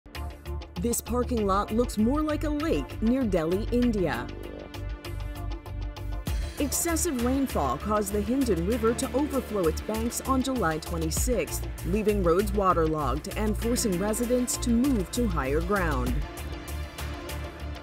Cars Sit in Flooded Parking Lot in India
A parking lot near Delhi, India, looks like a lake after a river overflowed its banks.